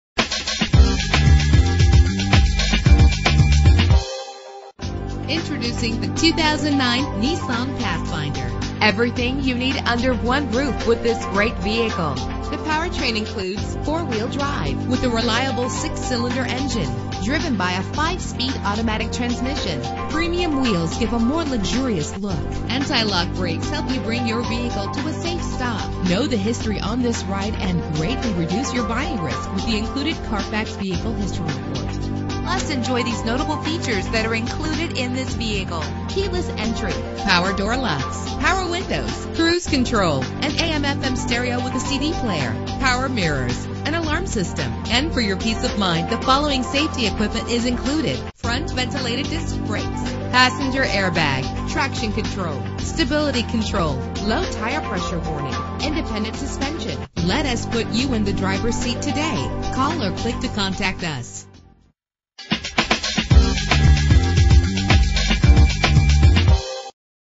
2009 Nissan Pathfinder - Bedford OH
Year: 2009
Make: Nissan
Model: Pathfinder
Trim: S
Engine: 4.0 liter 6 cylinder 24 valve
Transmission: 5-Speed Automatic
Color: Black
Mileage: 86054
Address:
11 Broadway Avenue